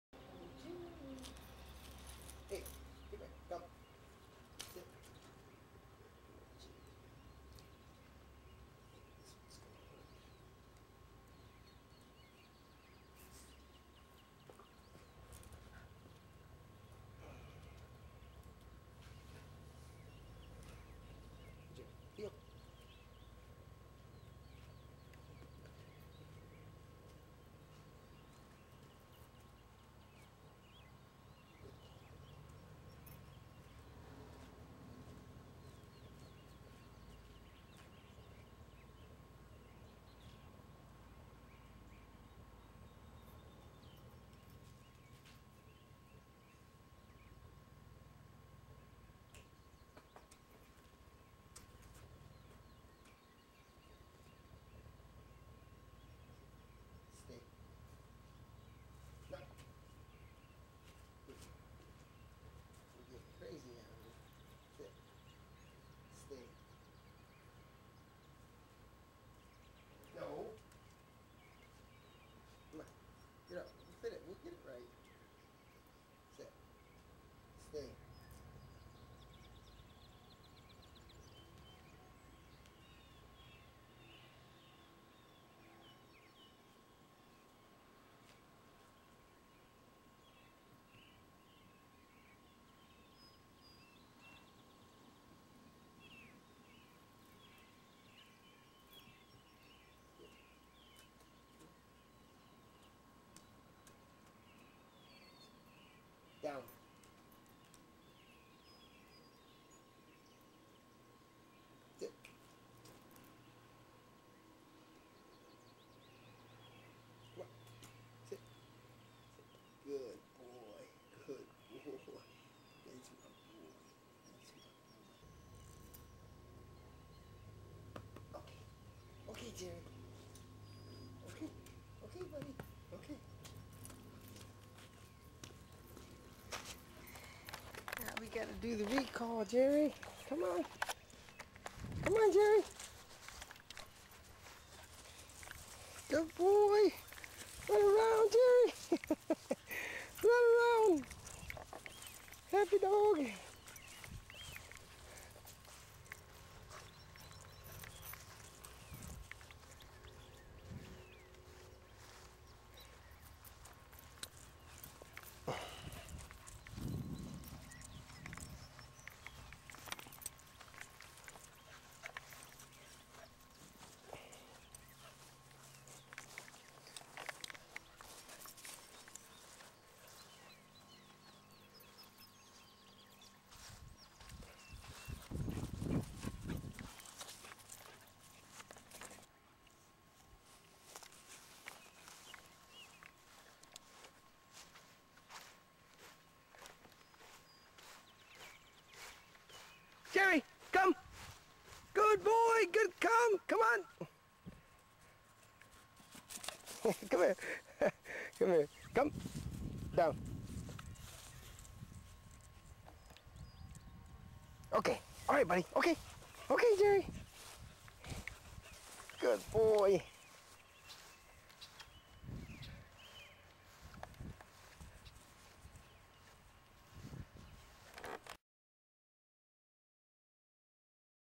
Basic obedience with 5 month old Jerry
Basic on lead obedience and Off lead come command with Jerry 5 month old Jerry (Golden Doodle)